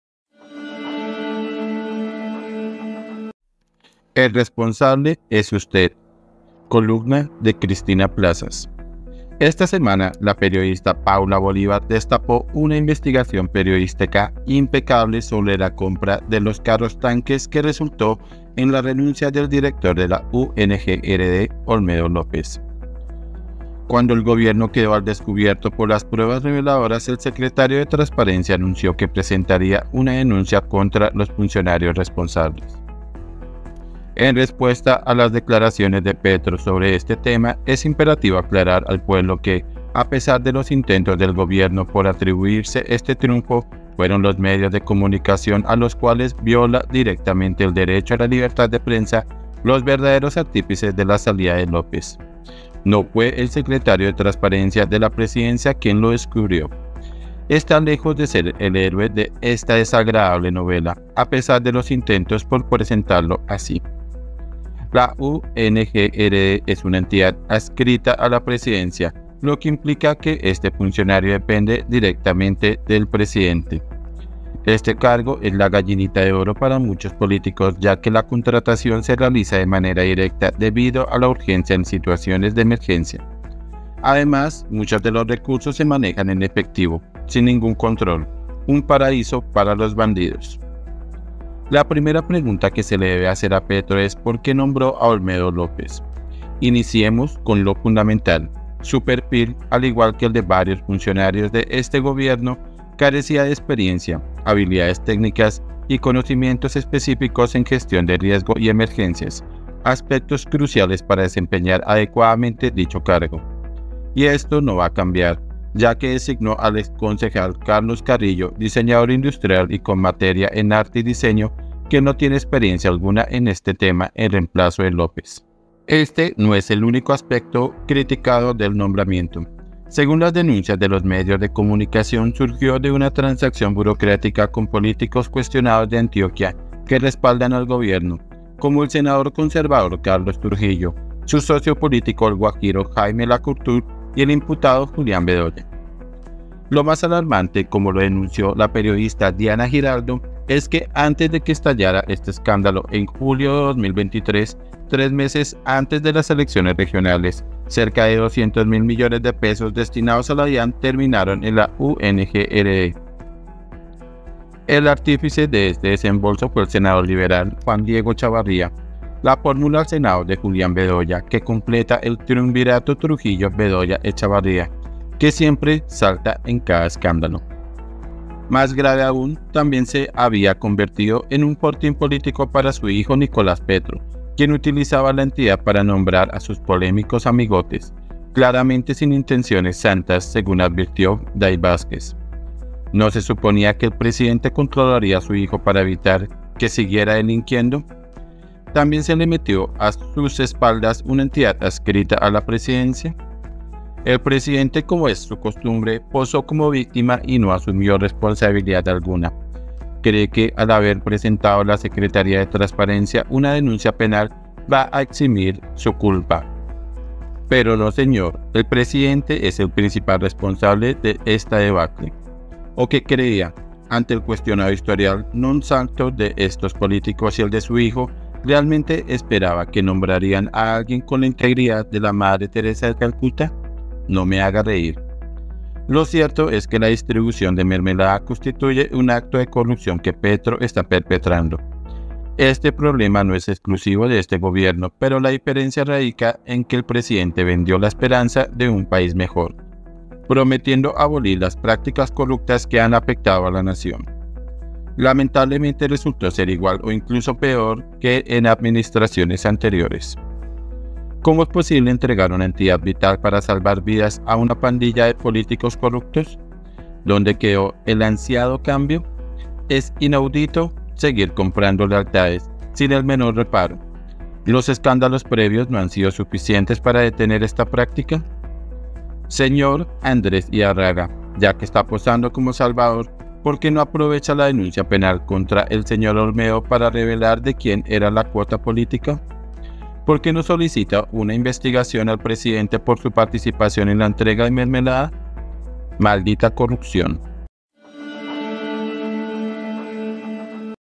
¡EL RESPONSABLE ES USTED! 🚨 Columna Cristina Plazas
Esta semana, la periodista Paula Bolívar destapó una investigación periodística impecable sobre la compra de los carros tanques que resultó en la renuncia del director de la UNGRD, Olmedo López.
columna CRISTINA LÓPEZ